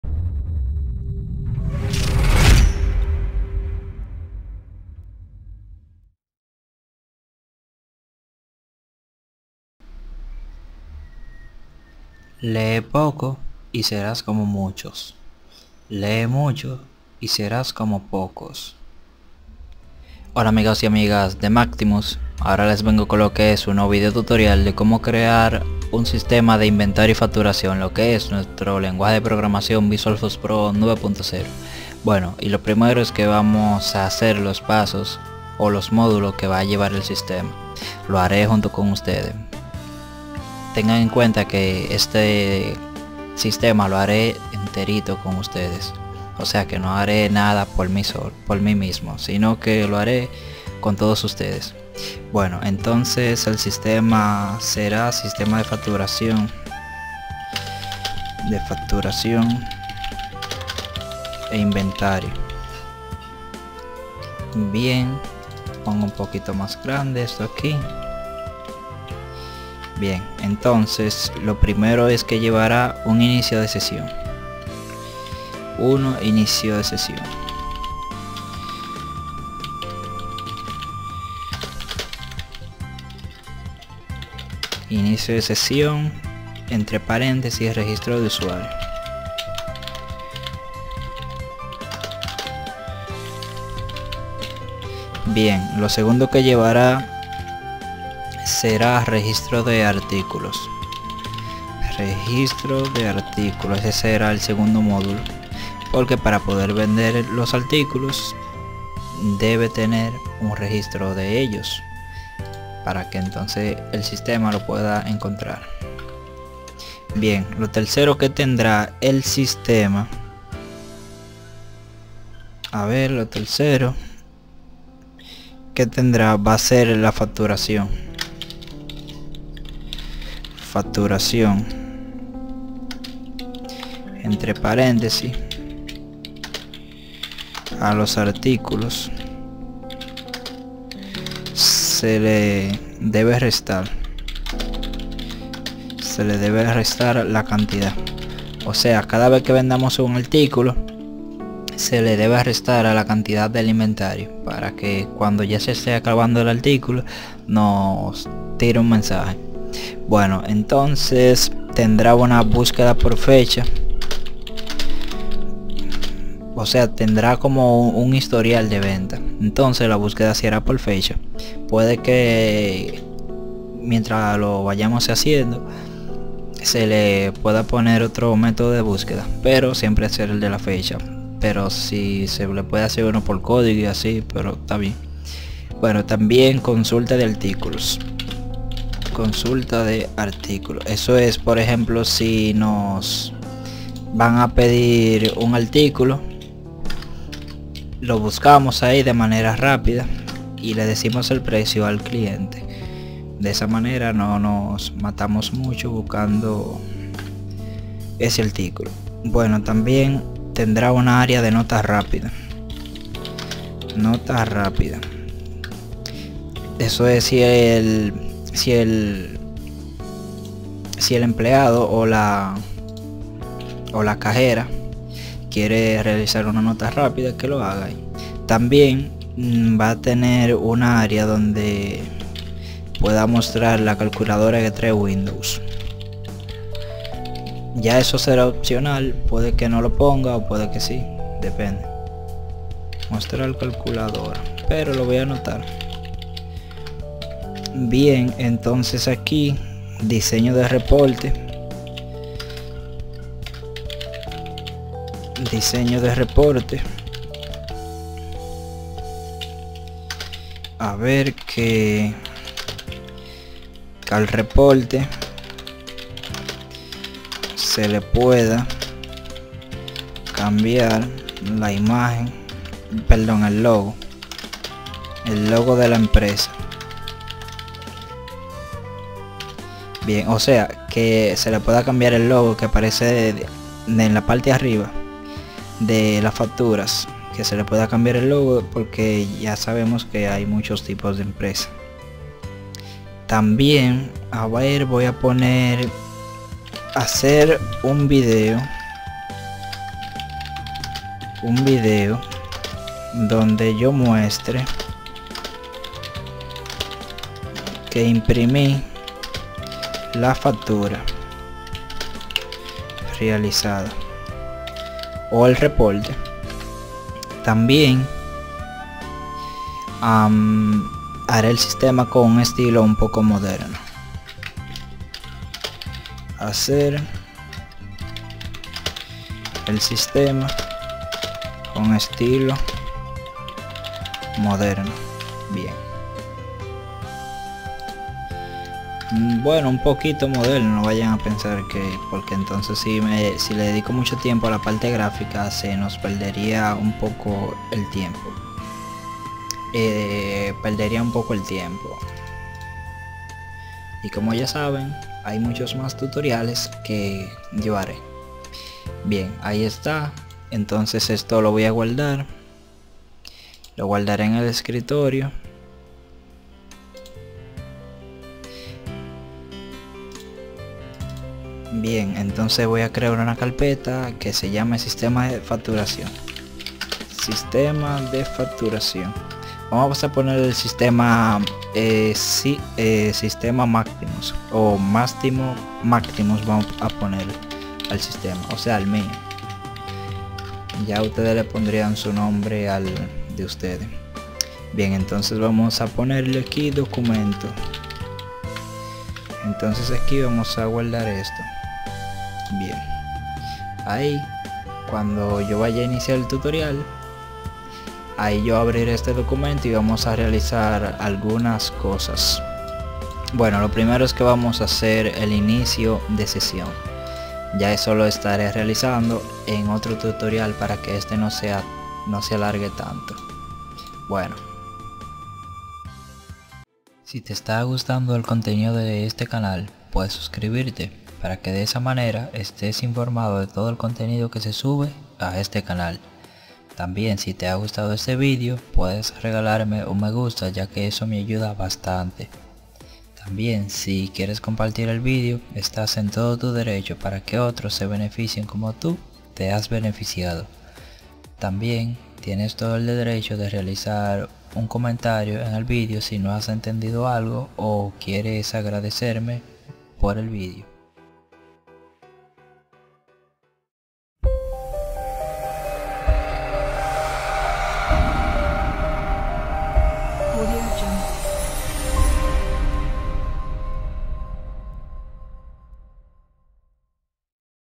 Como crear Sistema de facturación e inventario - Visual FoxPro 9.0 - ►VIDEO#1◄
Aquí estarás aprendiendo como hacer un sistema básico de inventario y facturación con Visual FoxPro 9.0.
Espero que te sirve de mucho aprender como hacer este sistema...

En este canal encontraras mucho contenido relacionado con la programación, diseño gráfico, manejo del computador entre muchas otras cosas mas.